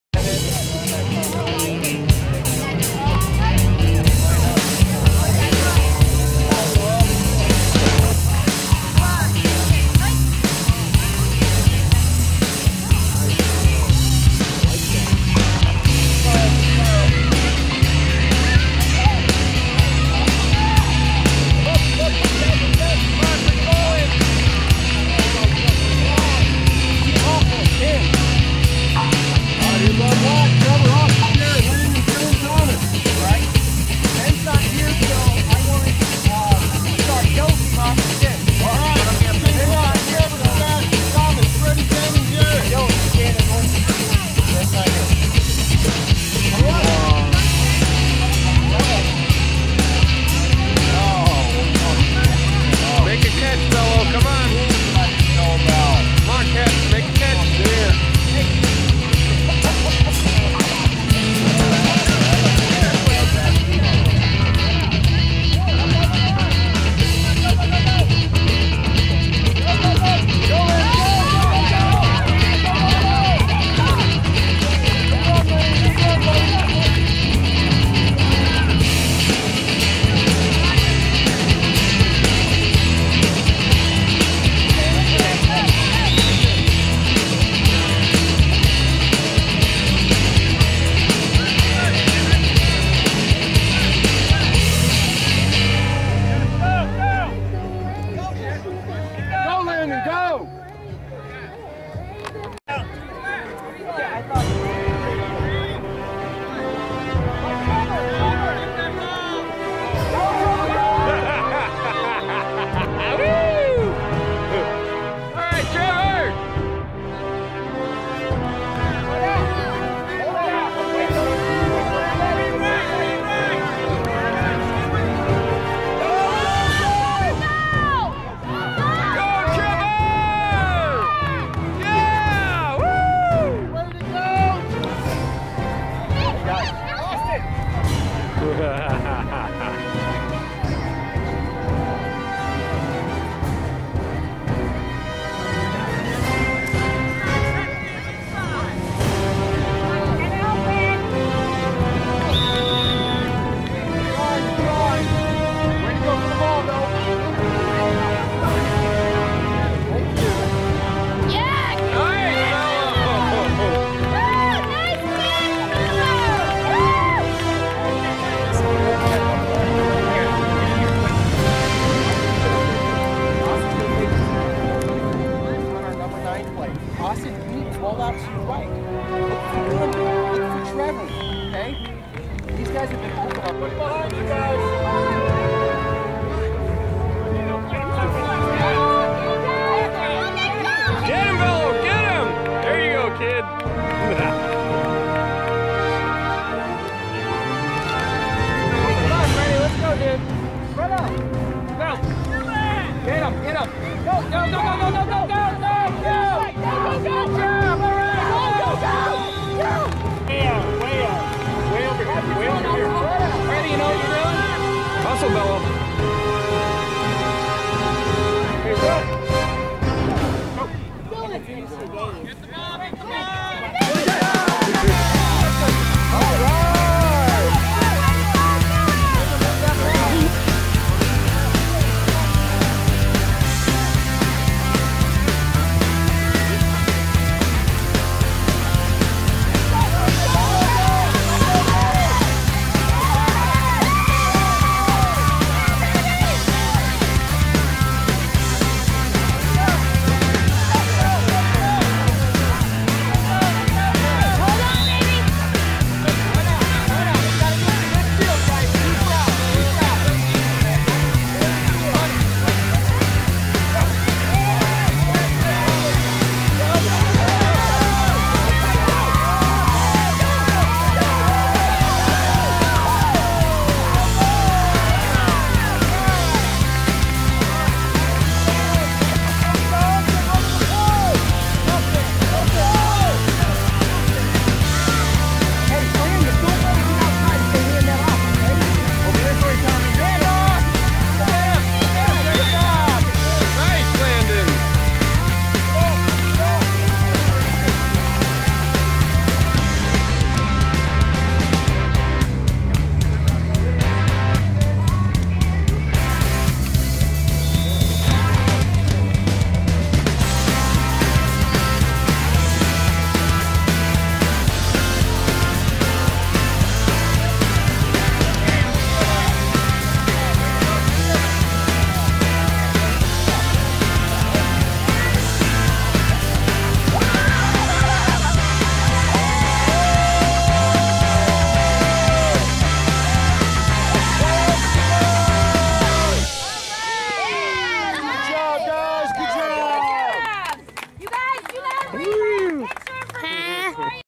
Flag Football 4 13 13
Some of Dylans plays from his Flag Football game.